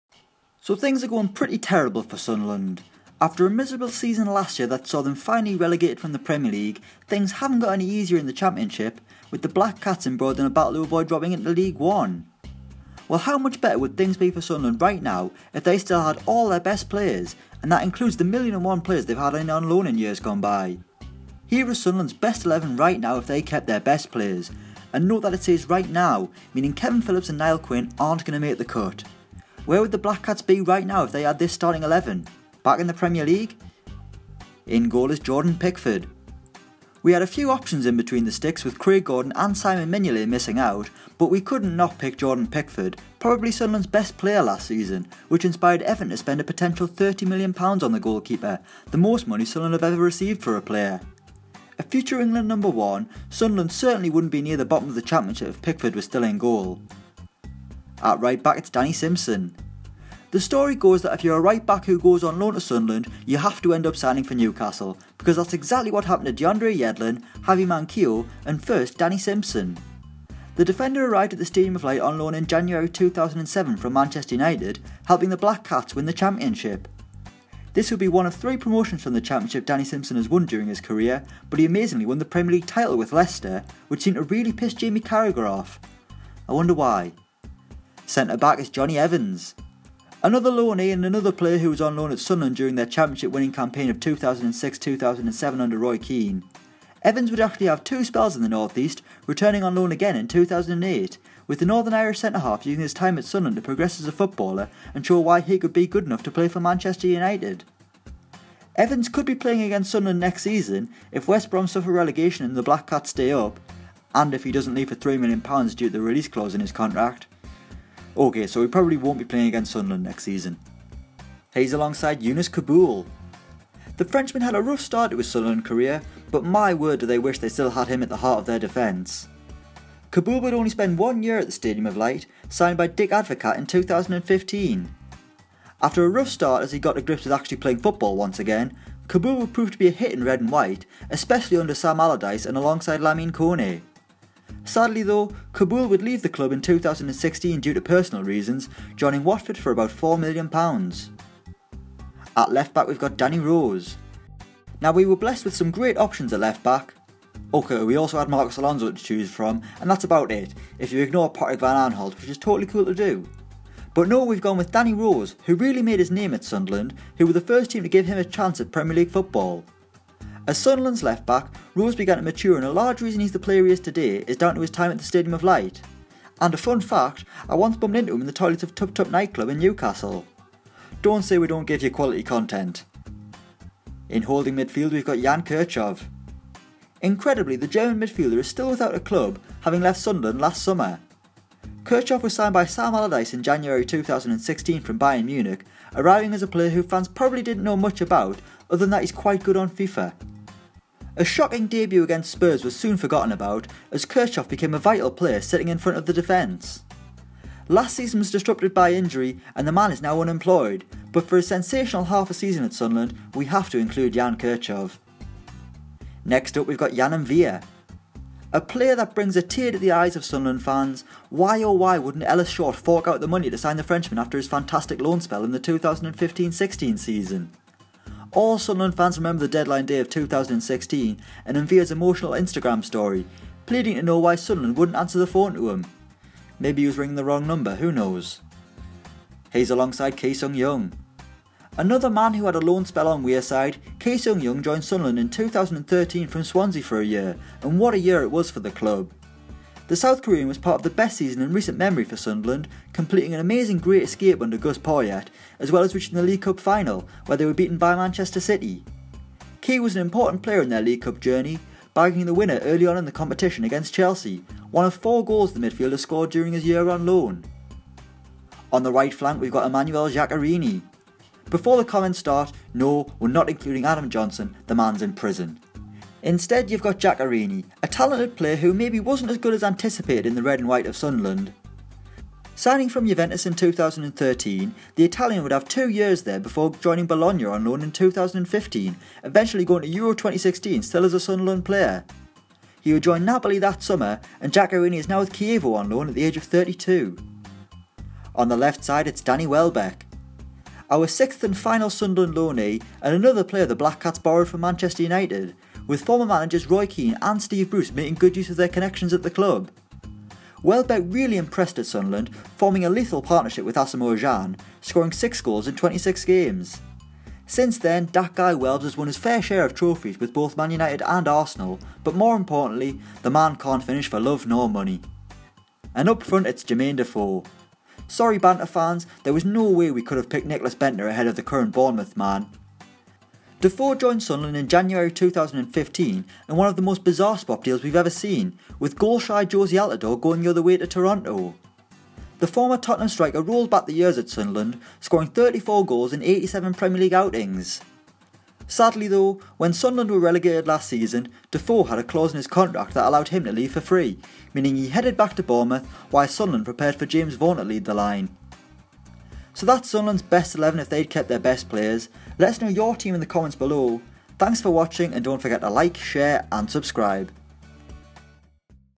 Sunderland XI If They Kept Their Best Players - Back In The Premier League?
Sunderland are having a torrid time, being relegated from the Premier League and now struggling in the Championship. How would the Black Cats be doing if they still had all their best players that they've seen come and go in years gone by?